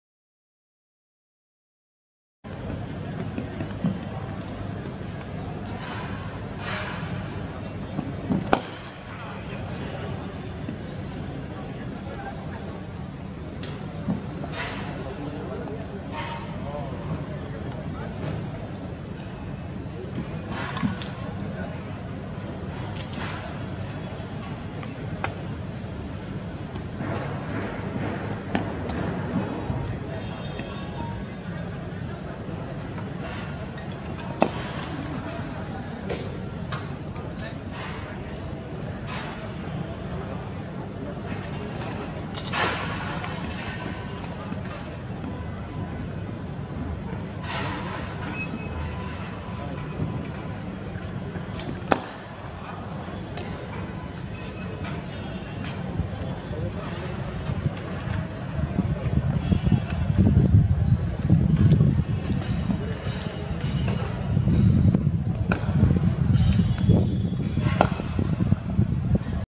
Ashwin and Jadeja against Pujara | SUBSCRIBE to CricketNews.com NOW!
Cheteshwar Pujara takes on Ravichandra Ashwin and Ravindra Jadeja in one of Team India's net sessions ahead of an important test match.

Three of India's important test players in action, through the eyes of our reporter, Sandipan Banerjee.